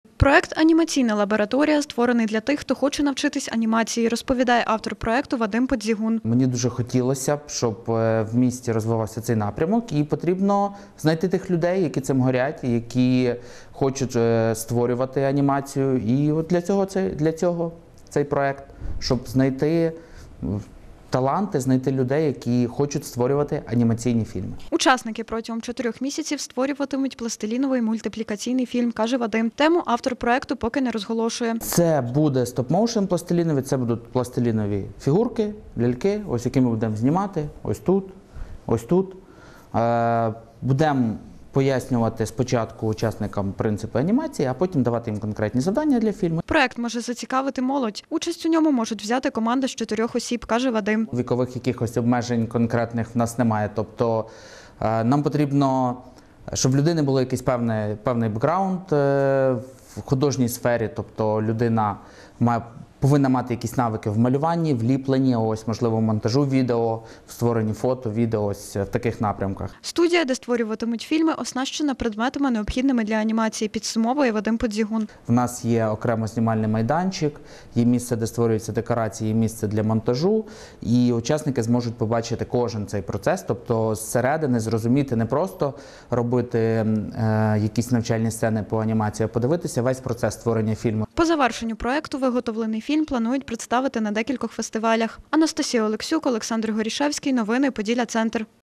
Реєстрація на проект для аніматорів розпочата
Розпочата реєстрація учасників на проект «Анімаційна лабораторія» у Хмельницькому. Про це розповідає керівник проекту Вадим Подзігун. У 2018 році його проект став переможцем конкурсу «Громадські ініціативи». За отриманні 60 тисяч гривень Вадим обладнав анімаційну студію. Зараз мультиплікатор шукає зацікавлених у анімації хмельничан.